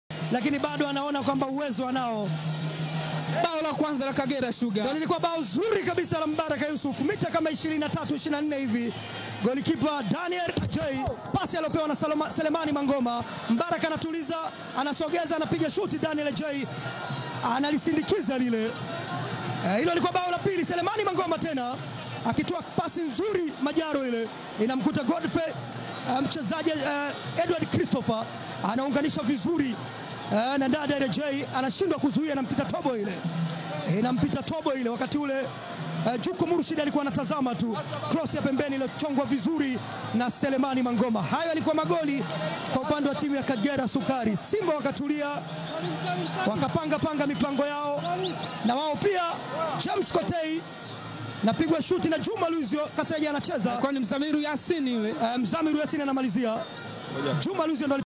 Magoli ya Kagera sugar vs Simba sc April 2, 2017 2 1
highlights za mchezo wa Kagera sugar VS Simba uwanja wa Kaitaba ambapo simba imefungwa magoli mawili kwa moja.